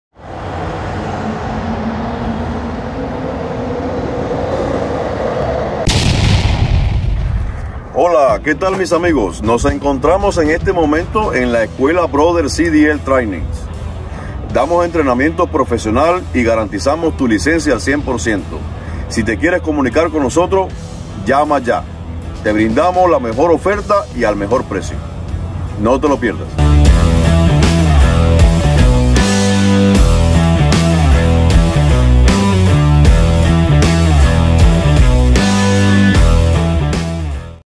OBTENER TU LICENCIA CDL EN TAN SOLO 4 SEMANAS LLAMA YA ESCUELA BROTHERS CDL TRAINING
COMO PUEDES TENER UNA LICENCIA CDL EN TAN SOLO 4 SEMANAS NO TE LO PIERDAS Y COMUNICATE PARA MAS INFORMACION AL
 - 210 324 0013 NOS PUEDES LLAMAR A CUALQUIER HORA GARANTIZAMOS TU LICENCIA...